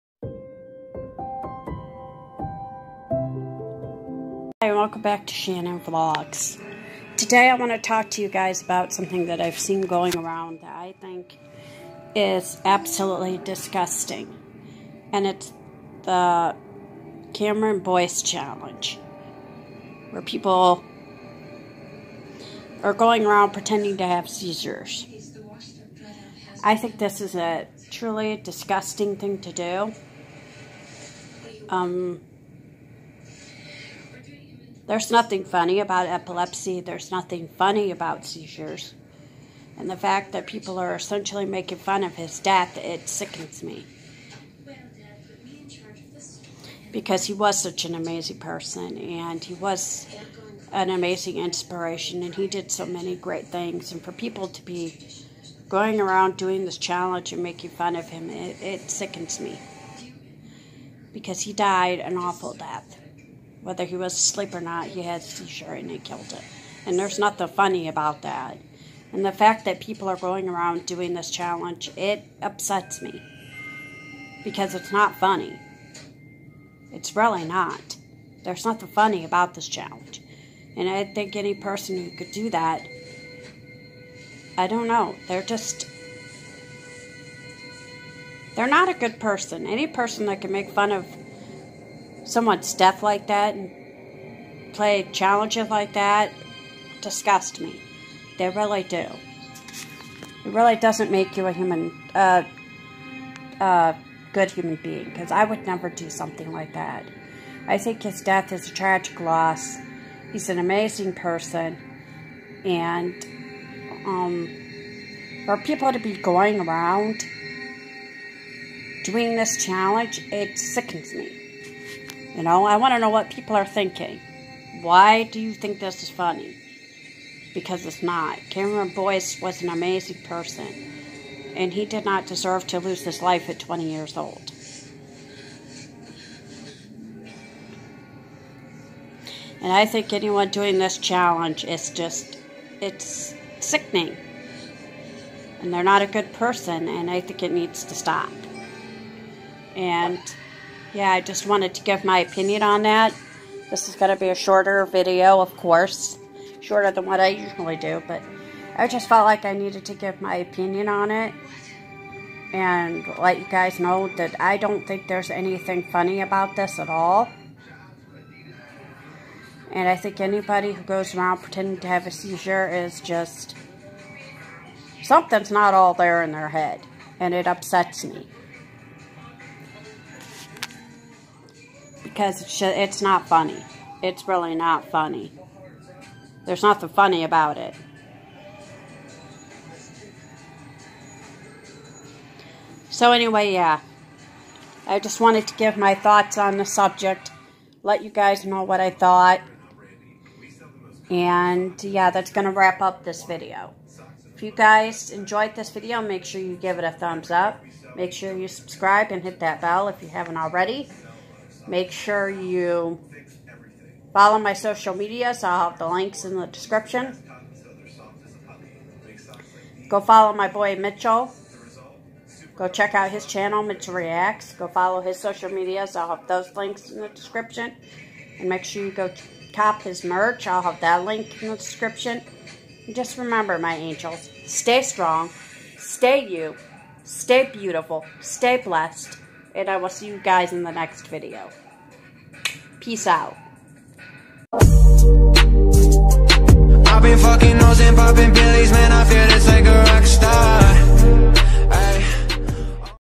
My reaction to the Cameron Boyce challenge!!!
Me giving opinion on the Cameron Boyce challenge.

My Links

Mitchell's Links

Mitchell's Merch